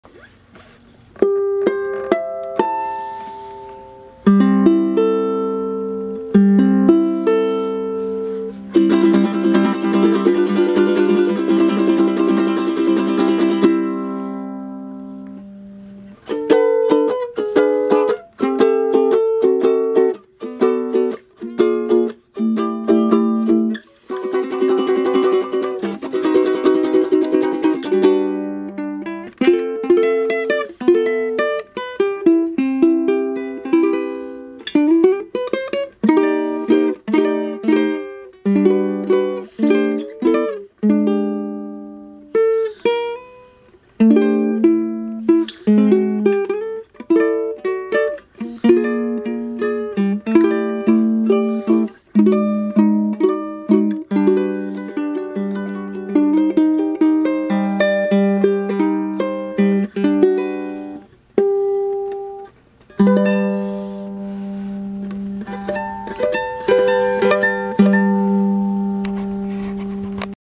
Compass Rose maple tenor sound clip
Antique sunburst flamed maple tenor sound smaple. This is a acoustic recording done with a simple digital camera no special recording. This ukulele also has a D-TAR acoustic pickup installed with external power pack.